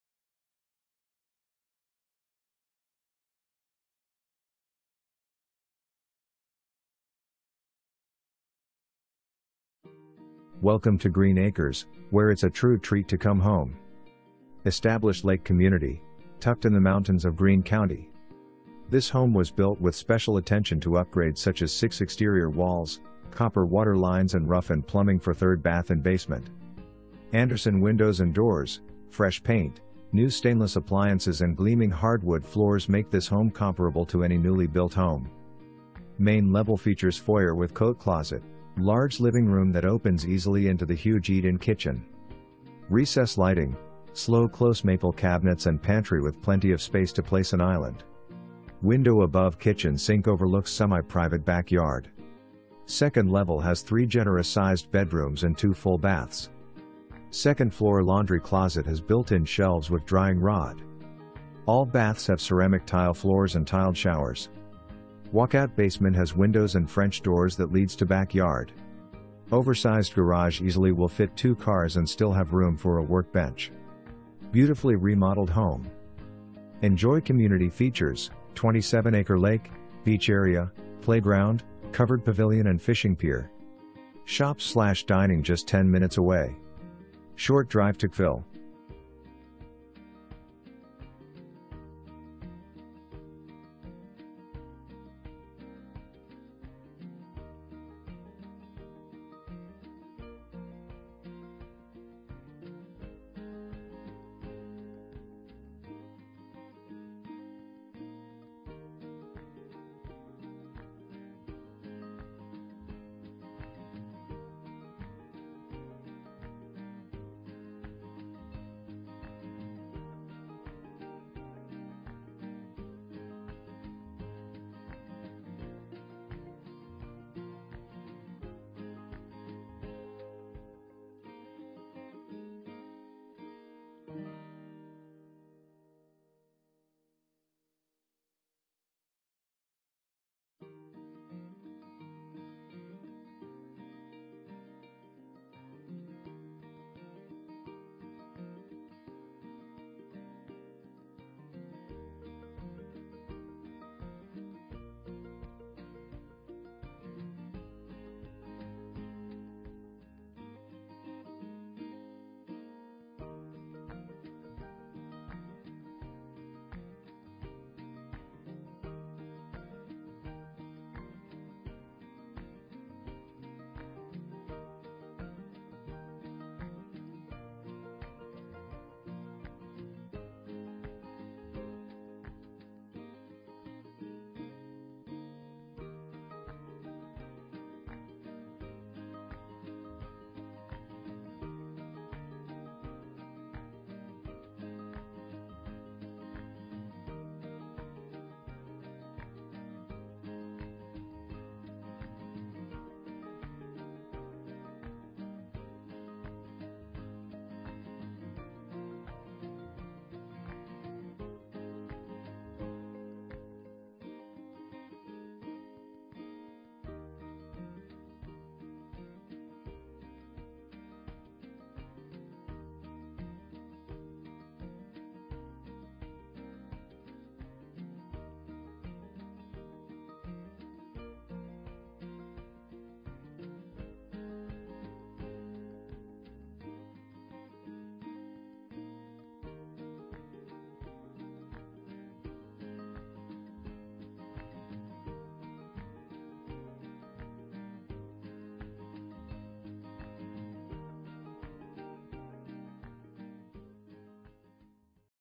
Homes for Sale - 693 N GREENE ACRES RD, STANARDSVILLE,
693 N GREENE ACRES RD
STANARDSVILLE 22973

Welcome to Greene Acres, where it’s a true “treat” to come home! Established lake community, tucked in the mountains of Greene County. This home was built with special attention to upgrades such as 6” exterior walls, copper water lines and rough-in plumbing for 3rd bath in basement. Andersen windows and doors, fresh paint, new stainless appliances and gleaming hardwood floors make this home comparable to any newly built home. Main level features foyer with coat closet, large living room that opens easily into the huge eat-in kitchen. Recess lighting, slow close maple cabinets and pantry with plenty of space to place an island. Window above kitchen sink overlooks semi-private back yard. 2nd level has 3 generous sized bedrooms and 2 full baths. 2nd floor laundry closet has built in shelves with drying rod. All baths have ceramic tile floors and tiled showers. Walk out basement has windows and French doors that leads to back yard. Oversized garage easily will fit 2 cars and still have room for a workbench. Beautifully remodeled home! Enjoy community features; 27acre lake, beach area, playground, covered pavilion and fishing pier. Shops/dining just 10mins away. Short drive to Cville.

2 full bath, 1 half bath

Trey Durham
Keller Williams, Savage & Co. Realtors